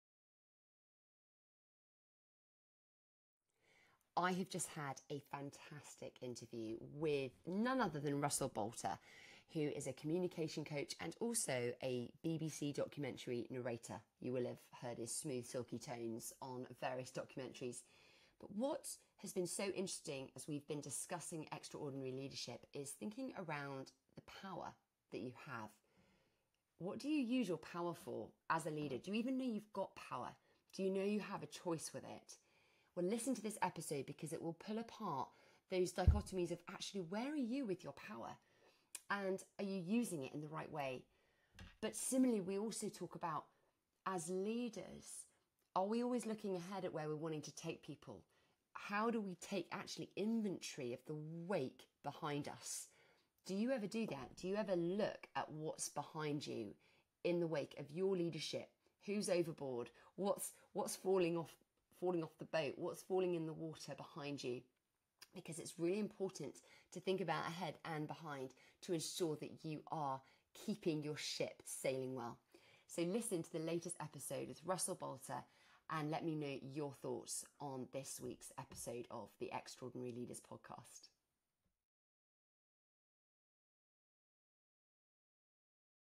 How To Make It Safe To Fail With Russell Boulter
Jim Collins, in his book Good To Great: Why Some Companies Make the Leap... and Others Don't, said to "be brutal with the facts but keep the faith."

I highly recommend this book, and I was reminded of it by none other than Russell Boulter, successful actor, director, and Founding Partner of GSB Comms, when he joined me on my podcast, Extra-Ordinary Leaders.

As soon as Russell shared this quote, I thought...there's definitely something there that fits with our exploration of how we can add something extra to our ordinary.

I think what it boils down to is the importance of creating a culture where it's okay to be brutal with the facts.

And as Russell so eloquently put it, “If you want to get good and stay good, you have to be willing to hear the brutal truth.”

How do you create a culture in your business that reassures people it's okay to share the facts? And how do you keep the faith?

Let me know - I'd love to hear from you!

If you'd like to hear the full conversation with Russell Boulter, it's available

to read the blog I wrote based on our insightful conversation.

Dolly